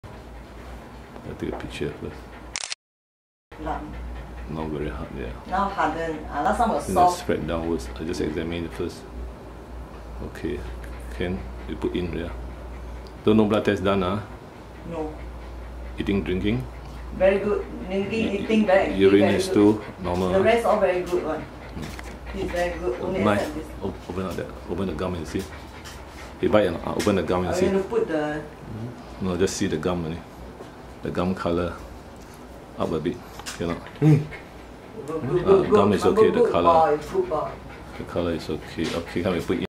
Lacrimal sac swelling Part 1
A big tense swelling below the right eyelid keeps enlarging. A lacrimal cyst, abscess or tumour?